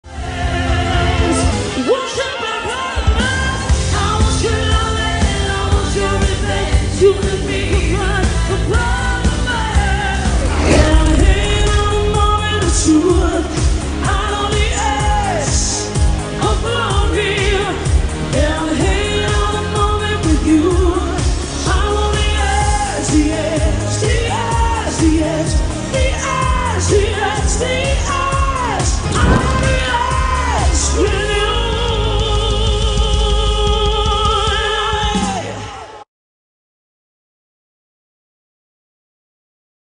Icons on Tour : Bonnie as Lady Gaga
An amazing tribute to Lady Gaga by Bonnie with all the energy, power, looks and sound of this amazing Icon.